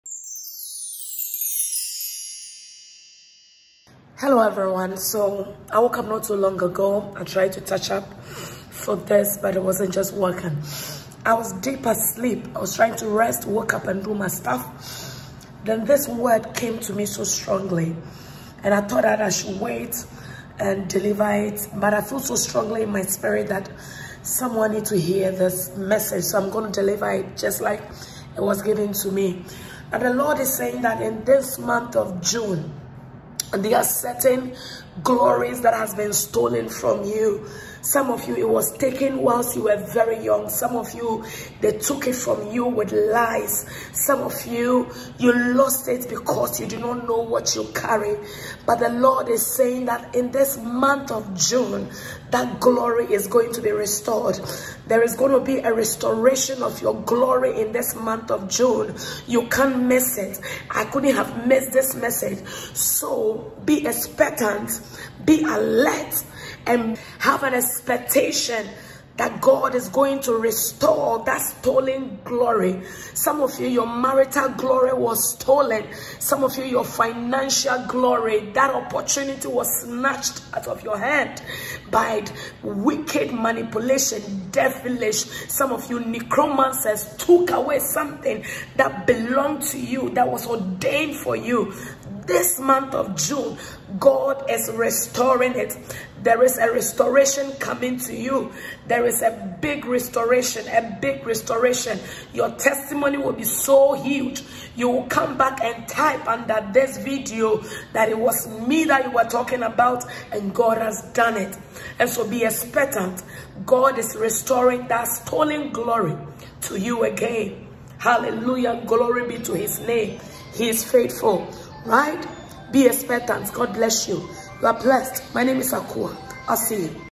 Prophetic Word for the month of June// Prophetess Akuwaa
This word came to me so strong in this month and God is causing a wind of restoration to blow through certain important aspects of your life. Be expectant. Be bless

You can join our midnight cross over prophetic prayers every day at 12:00 midnight for the month of June.